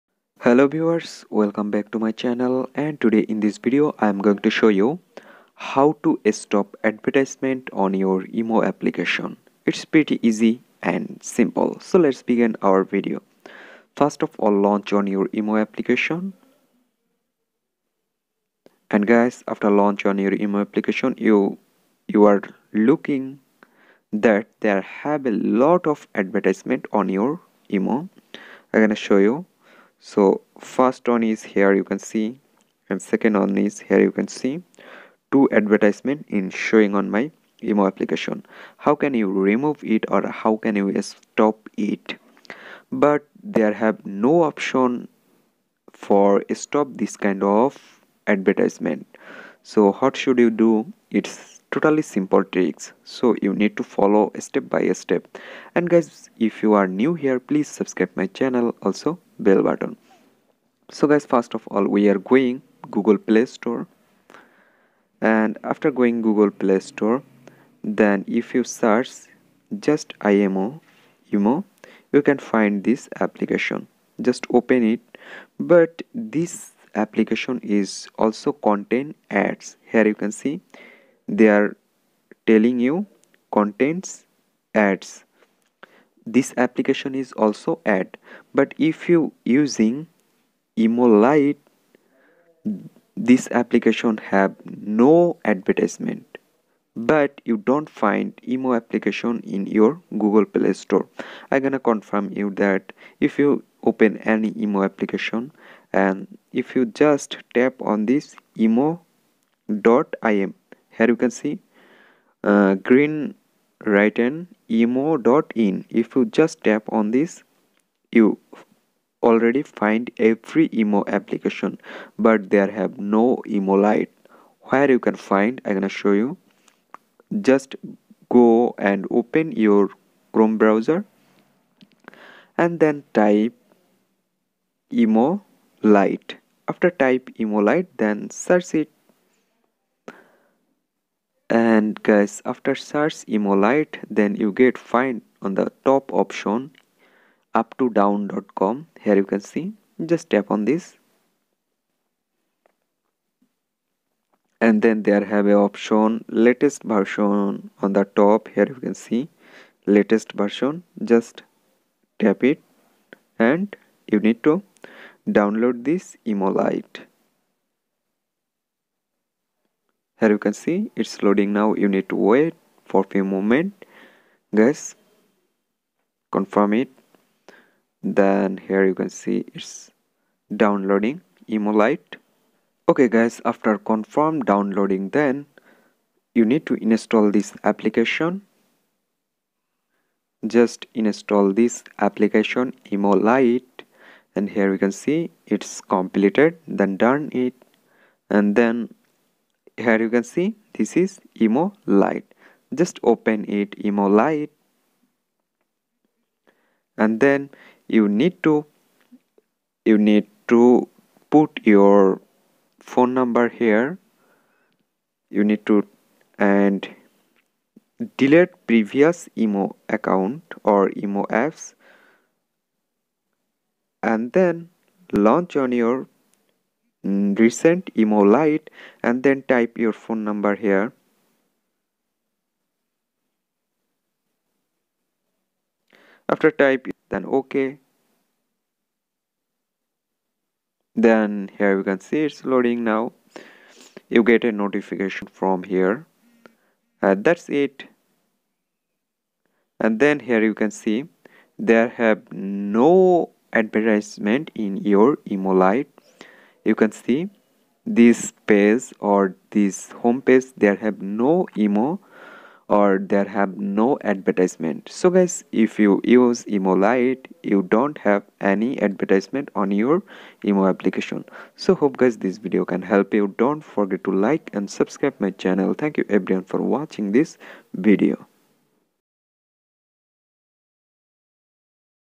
How To Stop Ads From Imo.
In this video i will show you how to stop ads from imo.